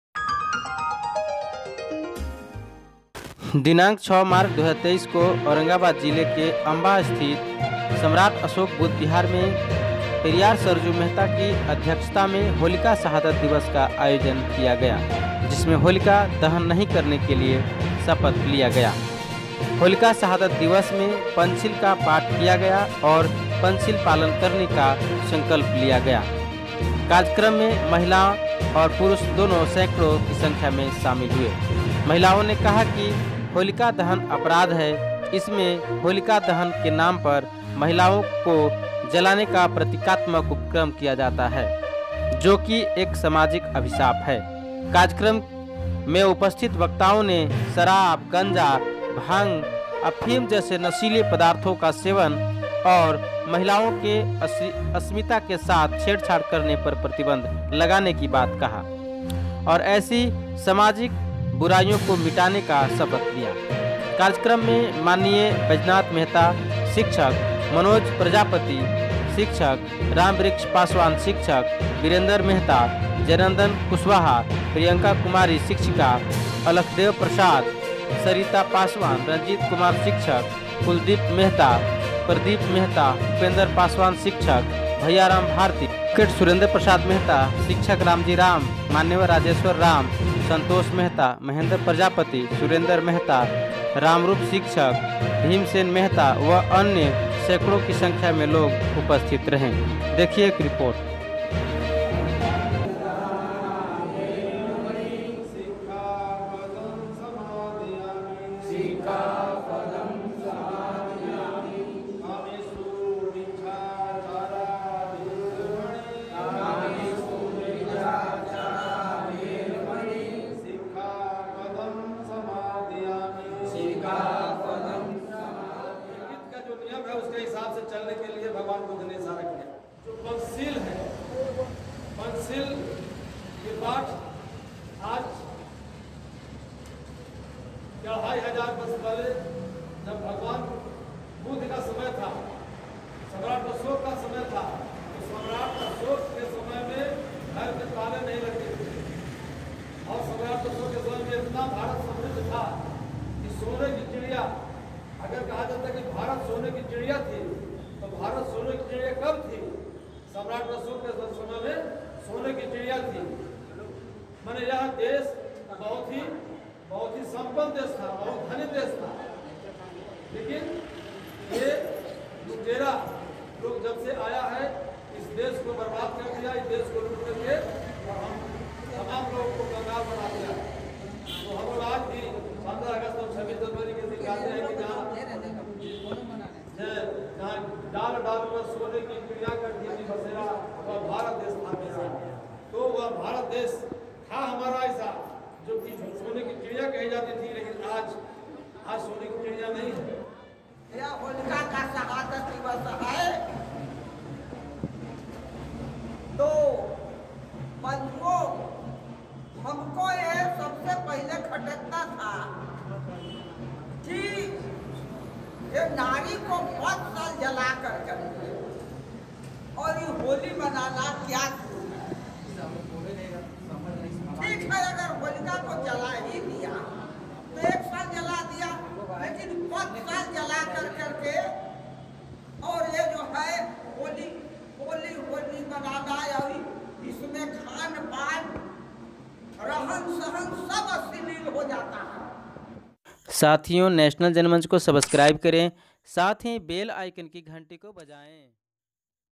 Amba में मनाया गया Holika शहादत दिवस | नारी दहन जैसा कुकर्म पर रोक लगाने कीअपील | National Janmanch
होलिका दहन एक समाजिक बुराई है और इस बुराई से समाज को मुक्ति दिलाना चाहिए इसी आशा के साथ 6 मार्च 2023 को सम्राट अशोक बुद्ध विहार , अम्बा में होलिका शहादत दिवस पर चर्चा-परिचर्चा का किया गया आयोजन। पुरा खबर के लिए विडियो जरूर देखें।